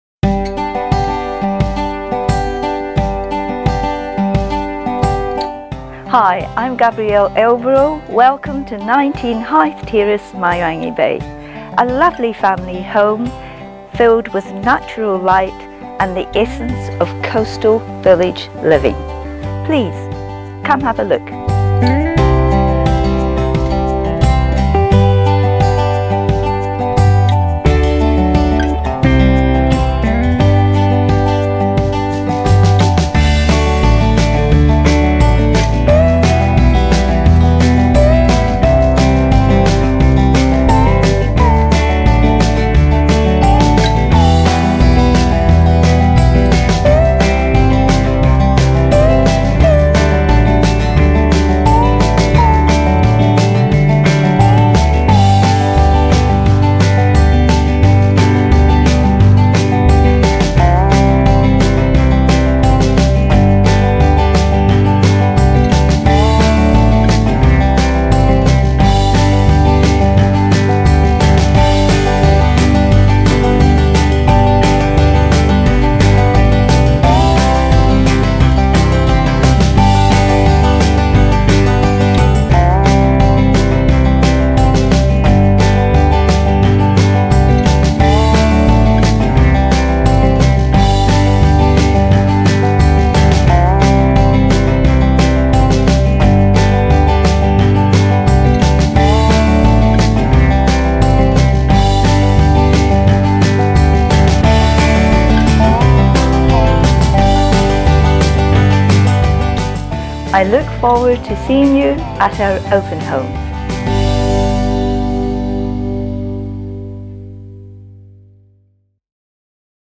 SOLD ~ 19 Hythe Terrace, Mairangi Bay - SOLD by Gabrielle Elborough
An elevated family home close to top schools, Mairangi Bay village and beautiful beaches.